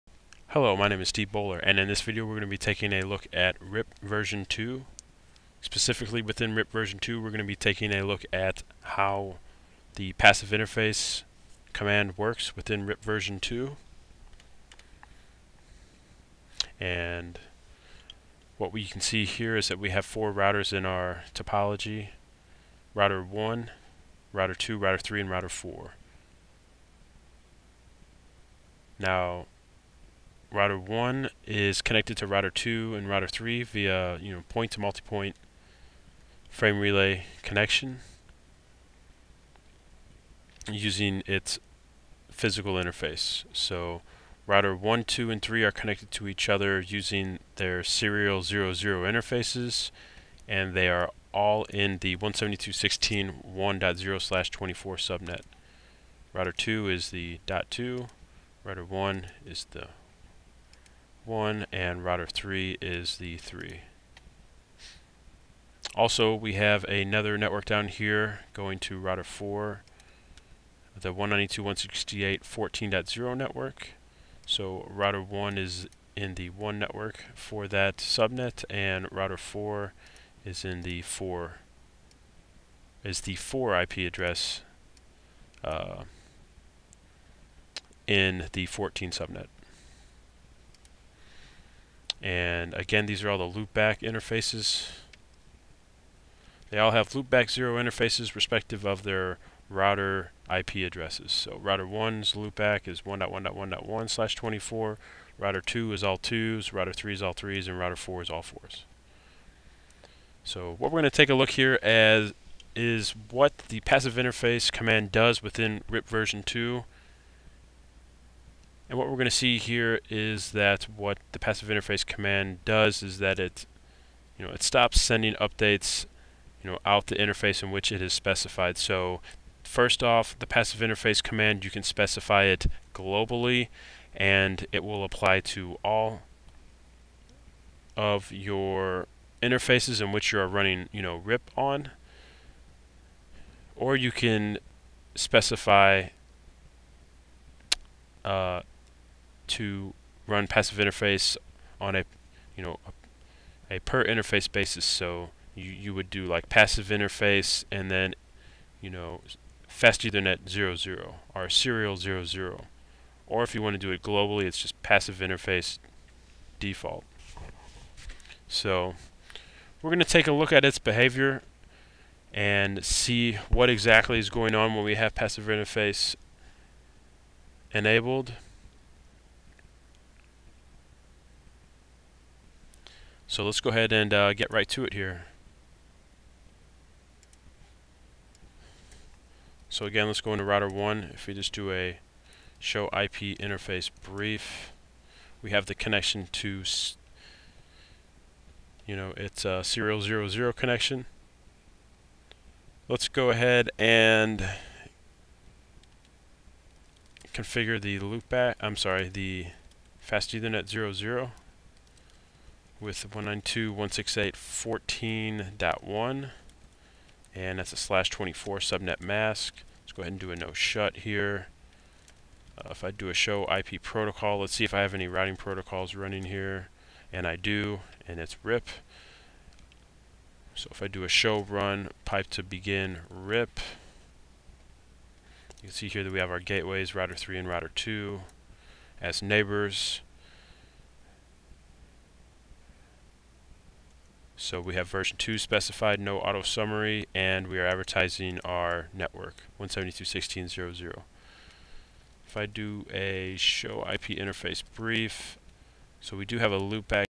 Routing Information Protocol RIPv2 Lab 4 Passive Interface
Download Cisco R&S Series 1 and 2 instantly from the link below and save 25%

In this video I configure the following:

* Configure/Explain RIPv2 concepts listed below and use show/debug commands for verification
* RIPv2 Passive Interface